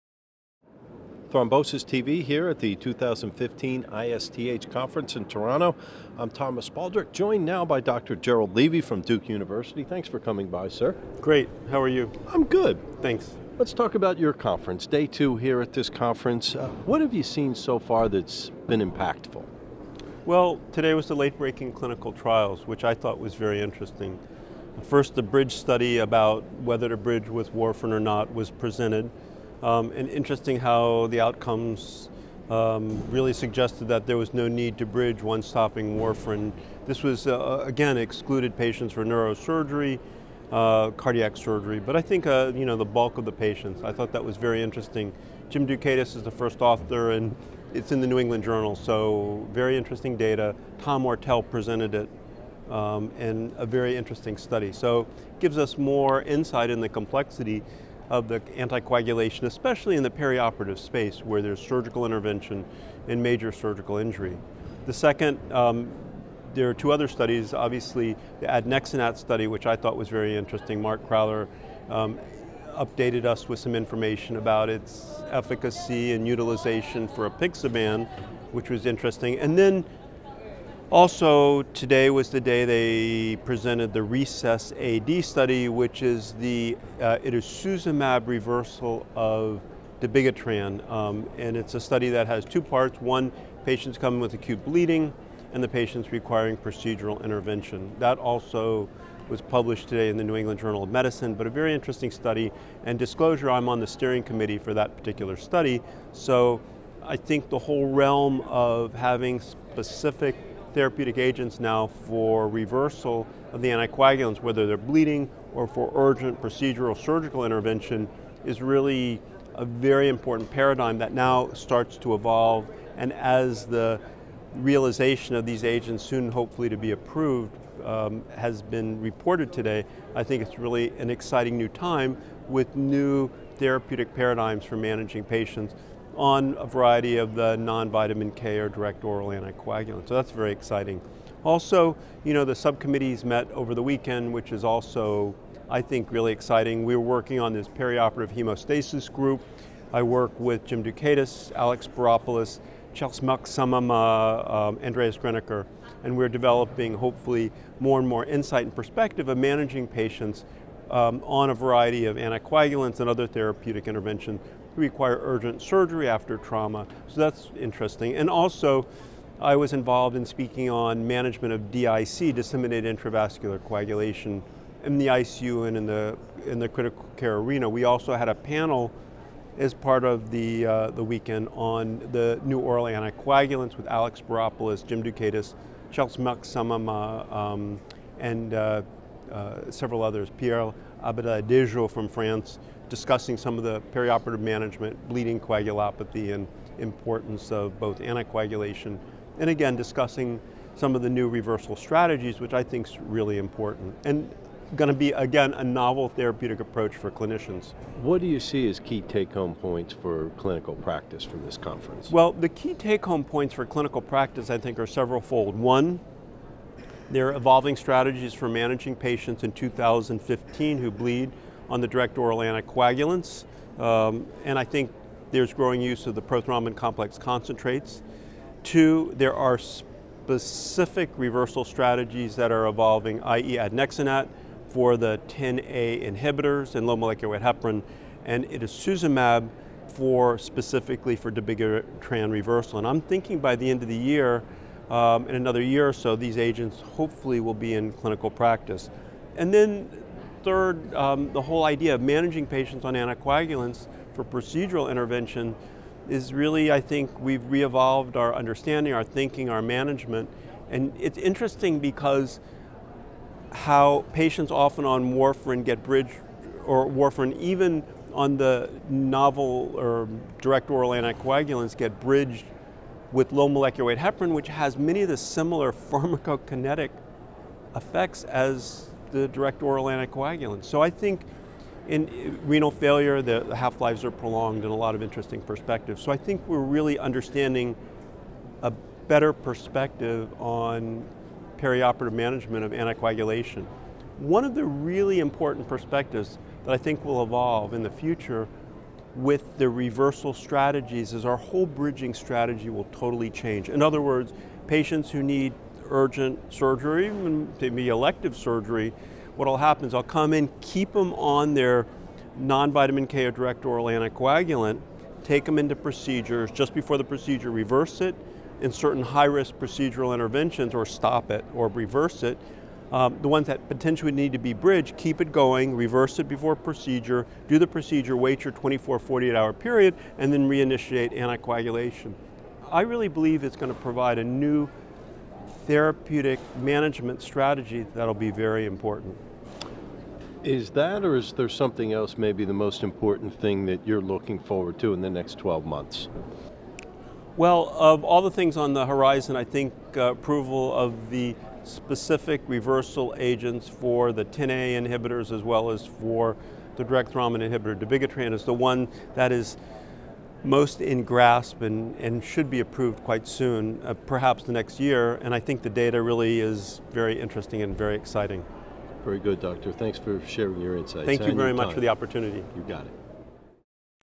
Key Points for Clinicians from ISTH 2015
Jerrold Levy, MD, of Duke University, discusses some of the late breaking clinical trial data and take-home points from ISTH 2015. The topics of greatest concern to clinicians in practice include: 1) There are evolving strategies for managing bleeding in patients taking direct oral anticoagulants, including the use of prothrombin complex concentrates, 2) the specific reversal strategies for Xa inhibitors and dabigatran are also evolving, 3) the approach to managing peri-procedural anticoagulation is changing due to the introduction of targeted NOAC reversal agents.